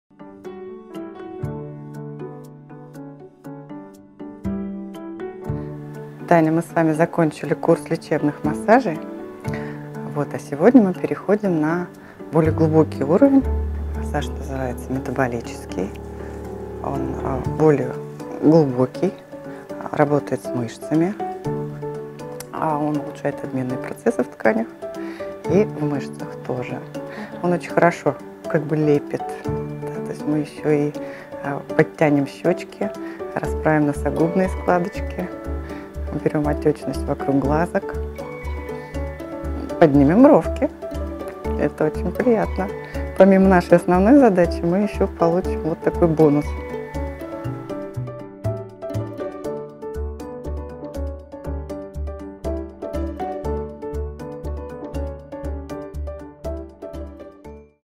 Проект "Постакне: успеть до свадьбы". Часть 4. Курс местной озонотерапии с массажем
Чтобы улучшить способность кожи к восстановлению Татьяне был проведен курс кислородо-озоновой терапия с массажем.  В случае с нашей героиней массаж сначала был лечебным, а затем метаболическим.  После таких процедур улучшается кровообращение и обменные процессы в коже, рубчики начинают быстрее выравниваться.

Команда проекта "Постакне: успеть до свадьбы":
Дерматовенеролог, дерматолог, косметолог, директор центра  - Корчевая Тамара Александровна;
Врач косметолог, дерматолог - Акопян Светлана Владимировна;
Косметолог-эстетист - Антипенкова Наталия Олеговна;
Врач невролог, гомеопат - Сапожникова Елена Сергеевна